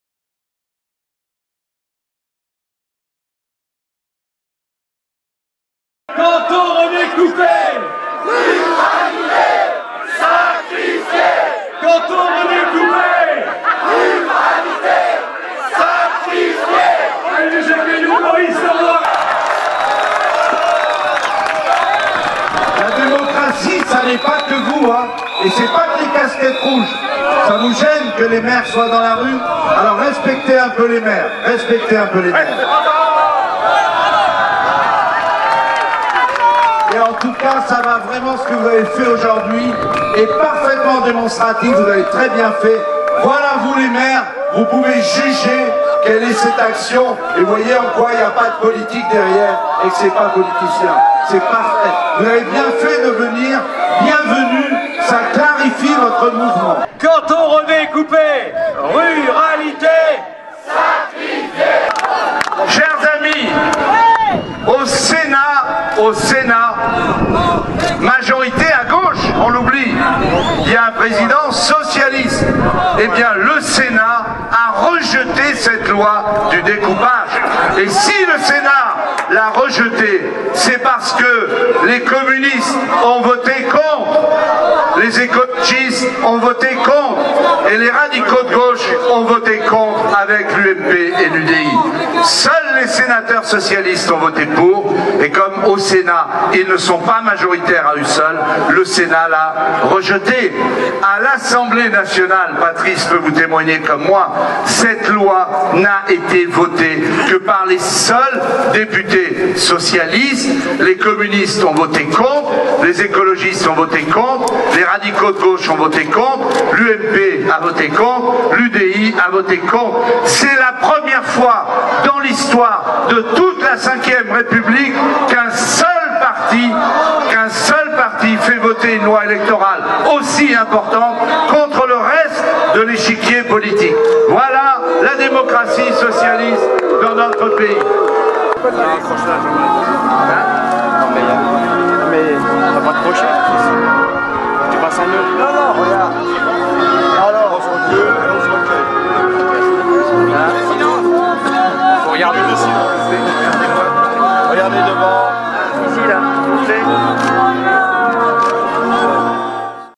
VIDEO. Blois : 500 voix s'élèvent pour défendre la ruralité
350 élus et plus de 500 personnes au total ont manifesté dimanche 16 février après-midi devant la préfecture de Loir-et-Cher. A l'appel de Maurice Leroy, président du conseil général, ils sont venus "défendre la ruralité menacée par le redécoupage cantonal". Pendant ce temps, le collectif "Sauvez les Carlines" se faisait entendre, en défendant le centre de séjour de Vars dont le conseil général a voté la fermeture...